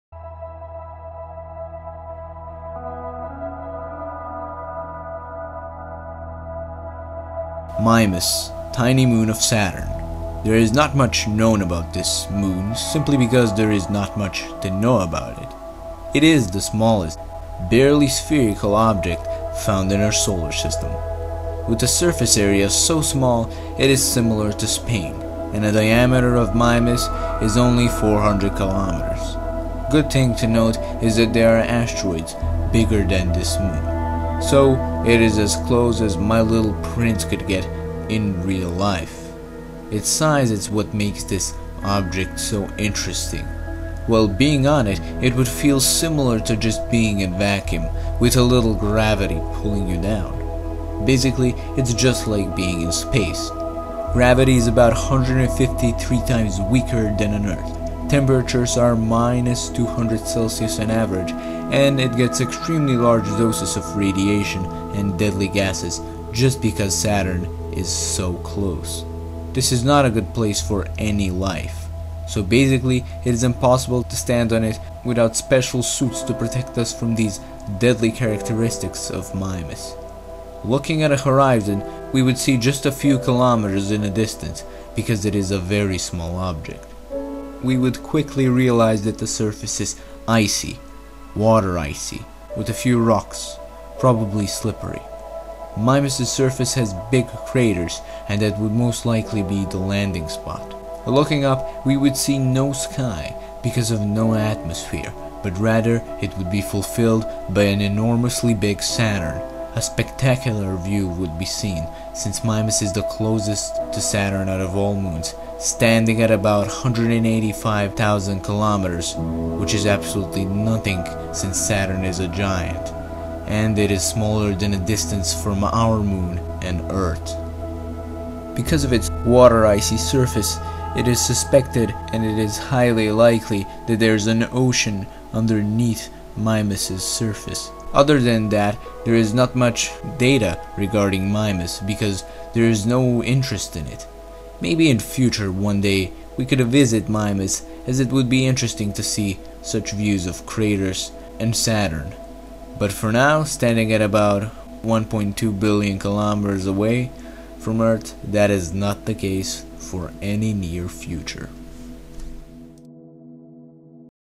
What Would Standing on Smallest Round Moon Mimas Feel Like?
What would it be like to stand right next to a gas gaint Saturn on a cold surface of the smallest spherical object Mimas? Watch the video to find out.